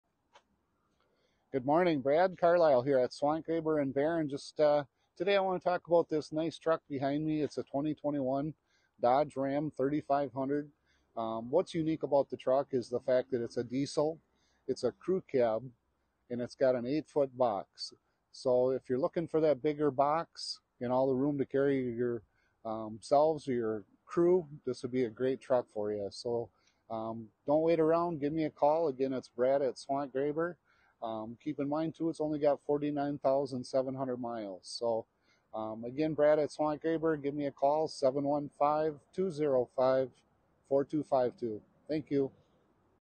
Ram 3500
This 2021 RAM Crew Cab 4x4 3500 Big Horn is a powerful and reliable motorcar. It has a 6.7L Cummins 6.7L Diesel Turbo I6 370hp 850 lb-ft engine, and a turbo diesel drivetrain that provides excellent power and torque. This car has driven 49,692 miles and features a bright white clear coat exterior and a black interior. Hill start assist and part time four wheel drive make this car an excellent choice for any driver looking for a reliable ride.